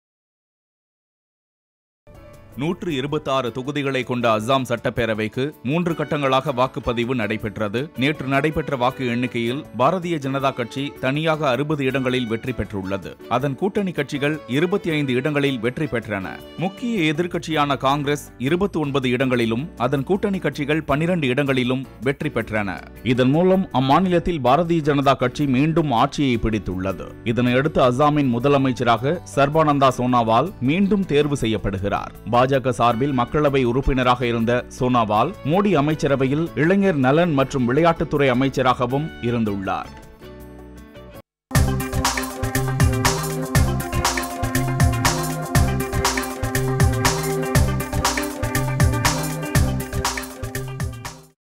அஸ்ஸாமில் மீண்டும் பாஜக ஆட்சி| Assam |BJP
சின்னத் திரையின் சினிமா இனிமே_இப்படித்தான் அஸ்ஸாமில் மீண்டும் பாஜக ஆட்சி| Assam |BJP

அஸ்ஸாம் மாநிலத்தில் மீண்டும் ஆட்சியமைக்கிறது பாரதிய ஜனதா கட்சி

பாஜக கூட்டணி 79 இடங்களில் வெற்றி பெற்றுள்ளது

காங்கிரஸ் கூட்டணிக்கு 50 இடங்கள் கிடைத்துள்ளன